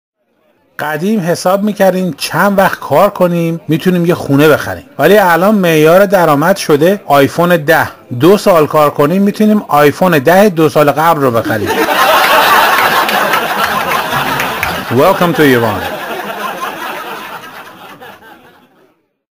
ESLAM’s جُک فارسی IPhone 10 in Iran آیفون در ایران
FARSI فـارسی   Party Jokes برای بـزرگسالان
کلیپ ساختن ساعتها وقت میبره ولی لایک زدن / شِر کردن 5 ثانیه
نکات خنده دارِ کمدین اسلام شمس برای پارتی ها
نامناسـب برای بی جنبه ها و جانماز آبکشها